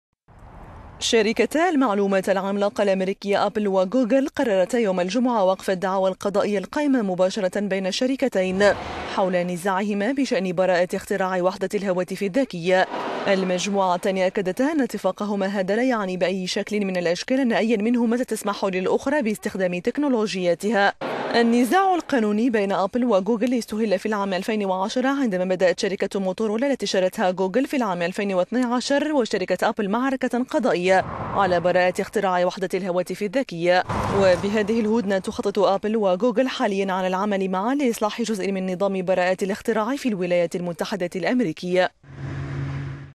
هدنة بين آبل وغوغل بشأن حرب براءات الاختراع
شرکتا المعلومات العملاقة الأمريكية آبل وغوغل قررتا يوم الجمعة وقف الدعاوى القضائية القائمة مباشرة بين الشركتين حول نزاعهما بشأن براءات اختراع وحدة الهواتف الذكية.

 المجموعتان أكدتا أن اتفاقهما هذا لا يعني بأي شكل من الأشكال أن أيا منهما ستسمح للأخرى باستخدام تكنولوجيتها.

النزاع القانوني بين أبل وغوغل استهل  في العام 2010 عندما بدأت شركة موتورولا التي اشترتها غوغل في العام 2012 مقابل  12.5 مليار دولار، وشركة أبل معركة قضائية على براءات اختراع وحدة الهواتف الذكية.

 غوغل سبق وأعلنت في العام الجا...